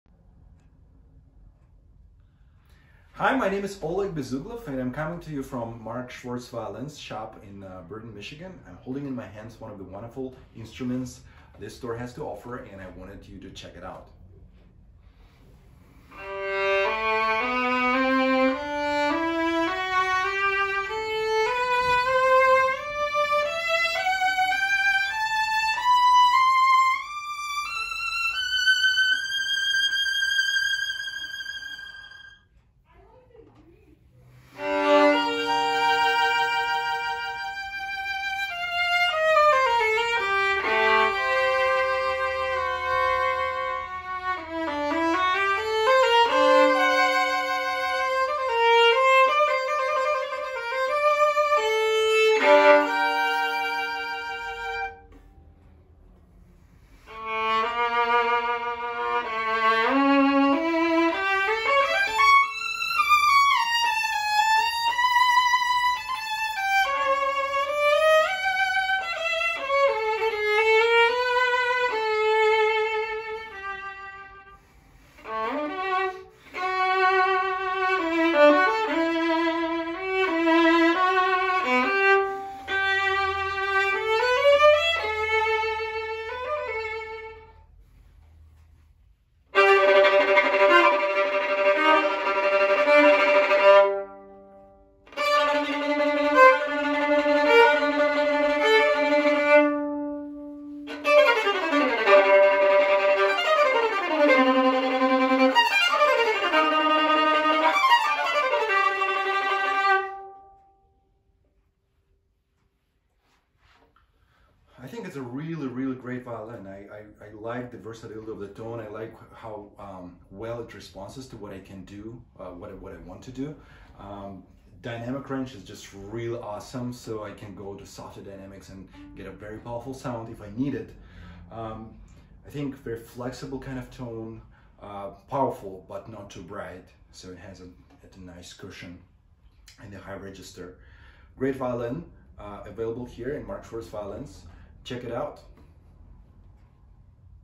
Violin s-2822, performed by Oleg V. Bezugloff
This Great Wall 3500 Violin has a two-piece, well-flamed back with matching ribs and scroll. It has a medium to wide grain spruce top. The violin is finished in a medium-red mahogany varnish. It has ebony fittings and Vision strings.